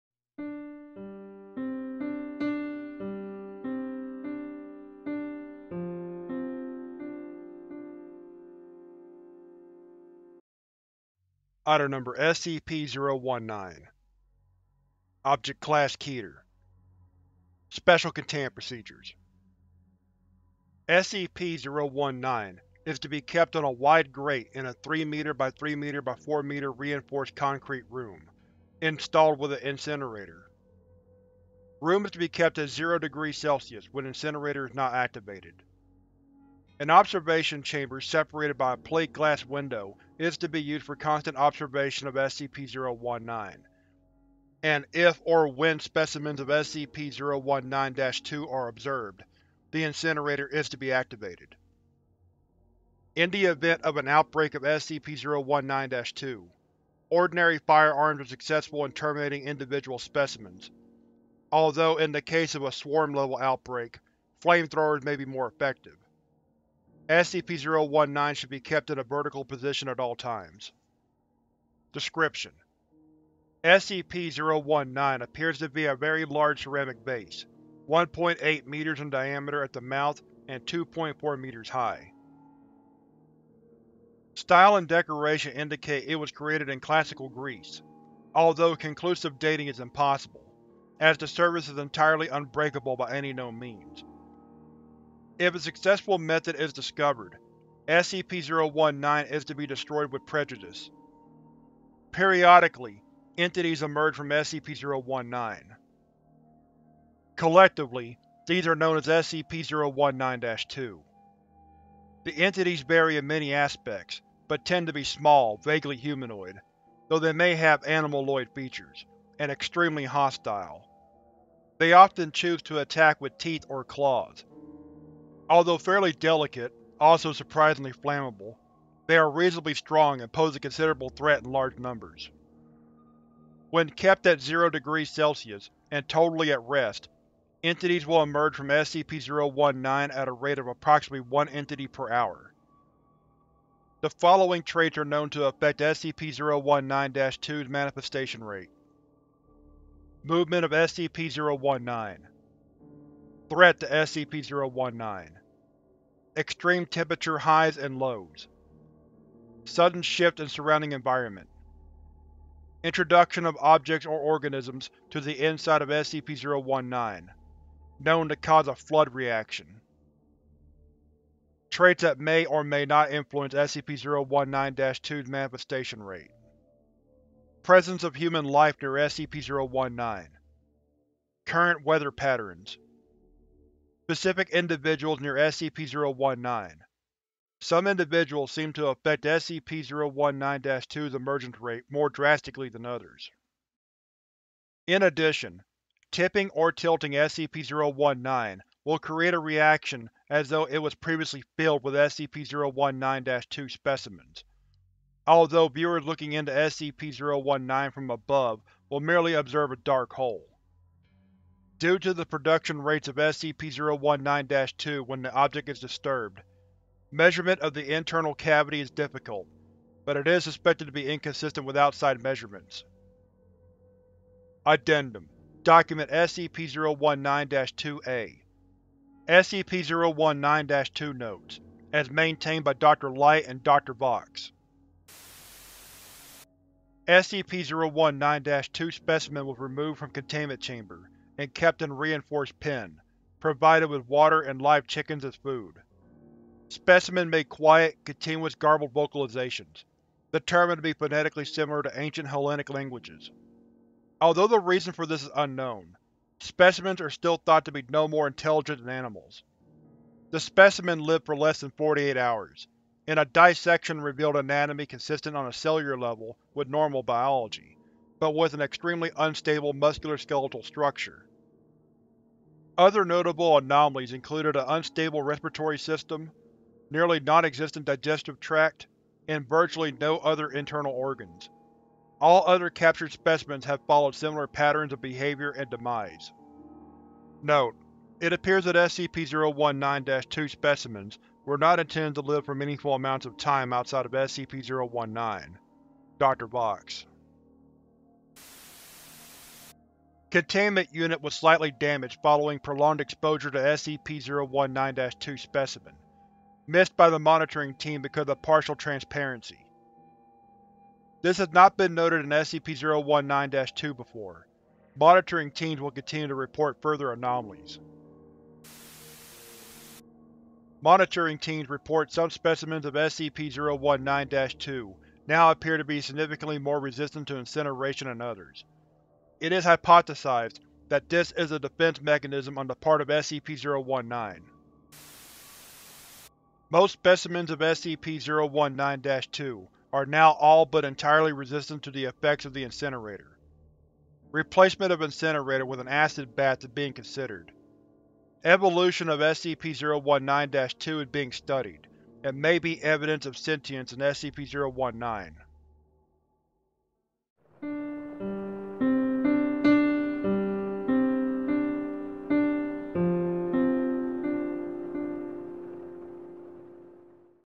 SCP-019 Monster Pot - Pandora's Pot: Ancient Vase Spawns Monster Horde
This SCP Foundation wiki reading is about SCP 019 "Monster Pot".  SCP-019 appears to be a very large ceramic vase, 1.8 m in diameter at the mouth and 2.4 m high. Style and decoration indicate it was created in Classical Greece, although conclusive dating is impossible, as the surface is entirely unbreakable by any known means. If a successful method is discovered, SCP-019 is to be destroyed with prejudice.

Periodically, entities emerge from SCP-019. Collectively, these are known as SCP-019-2. The entities vary in many aspects, but tend to be small, vaguely humanoid (though they may have animaloid features), and extremely hostile. They often choose to attack with teeth or claws. Although fairly delicate (also, surprisingly, flammable), they are reasonably strong and pose a considerable threat in large numbers.

Written by Sophia Light

If you wish to help contribute subtitles of a video into different subtitles please click here to begin helping out the show!